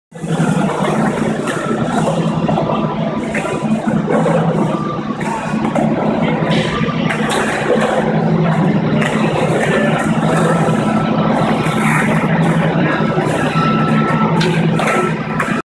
Marginal Way 1.31.15(1)
Max learns to work the bowl @ Marginal Way